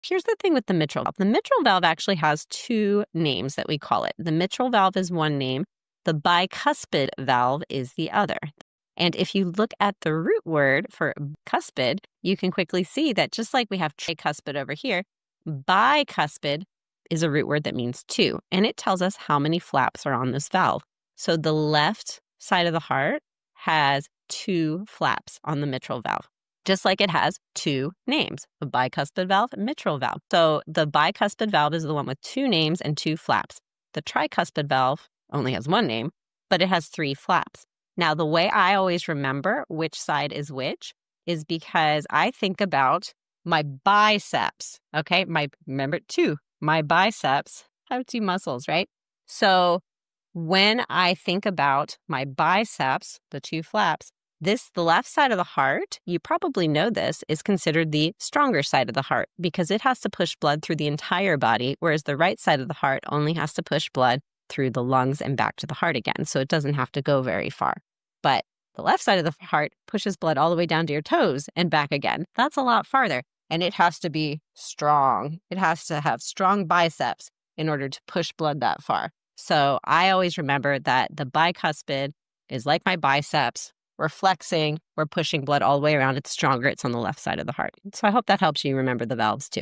Simple Trick to Remembering Heart Valves (That Actually Sticks)
Mitral valve? Bicuspid valve? They’re the same thing.

Here’s the trick: bi means two—so the bicuspid/mitral valve has two flaps on the left side of the heart. Think of your biceps (two muscles) to remember: left side = stronger side = bicuspid.

Simple connections like this make memorization unnecessary.